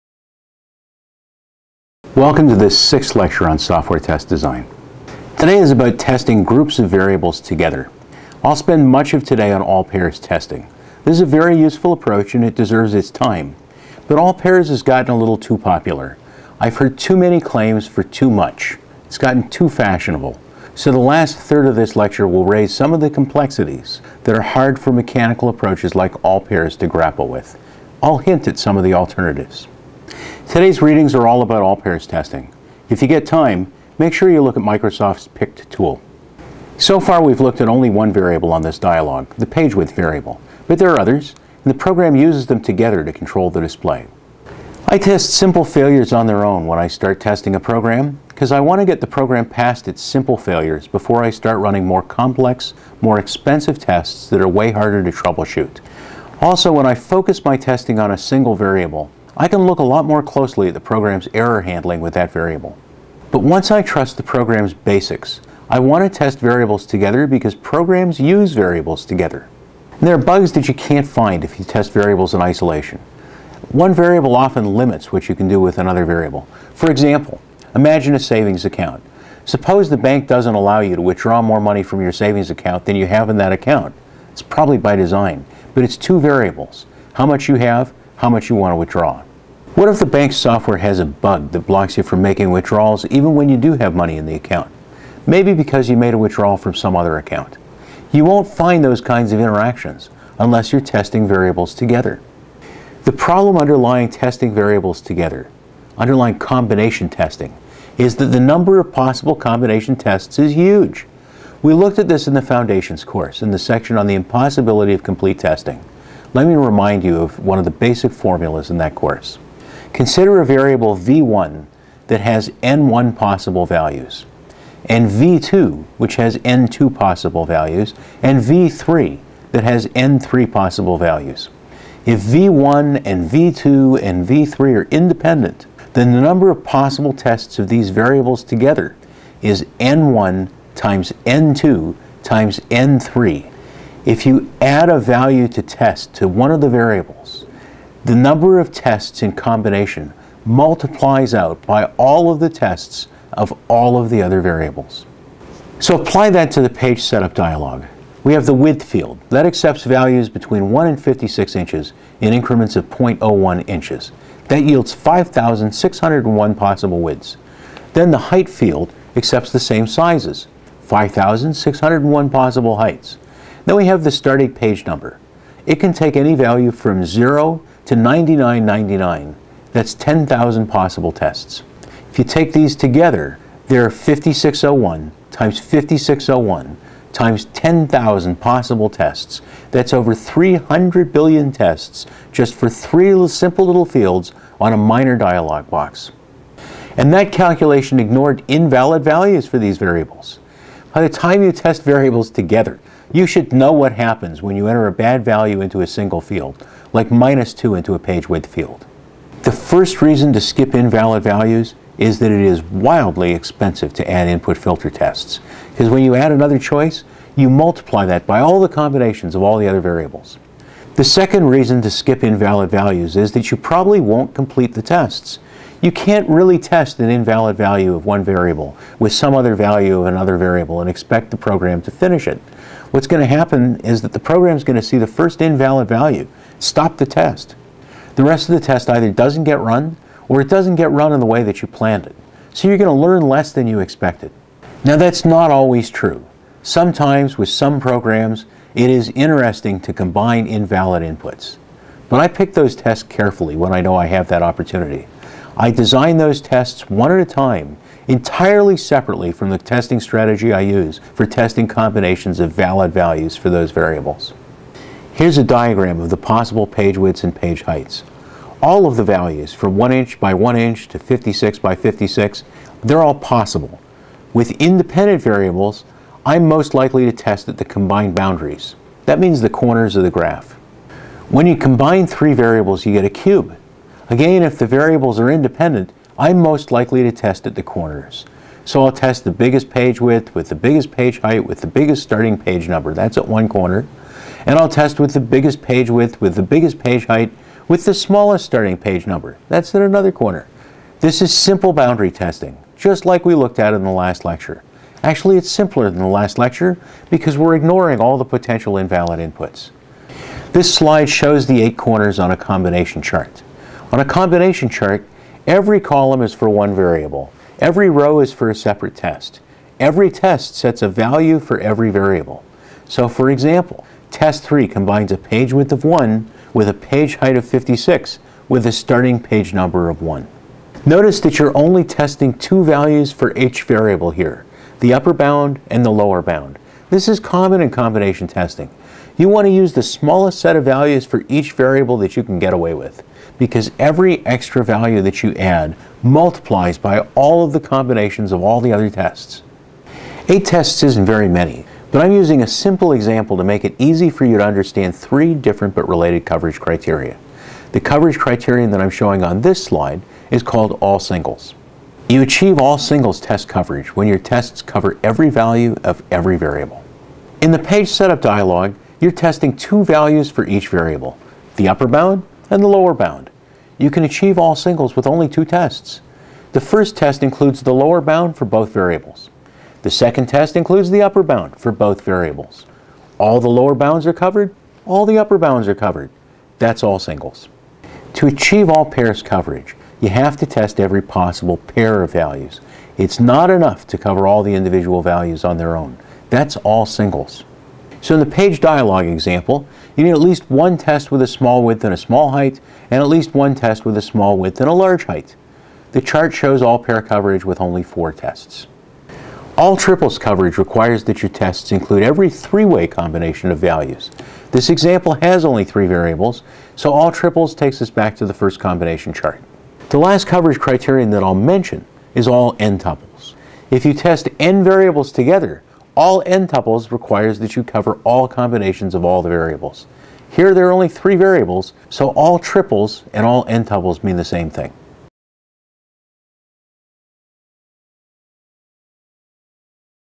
BBST Test Design 6A: Combination Testing
In this lecture, Cem Kaner focuses on ways to test several variables together, emphasizing combinatorial approaches (such as all-pairs).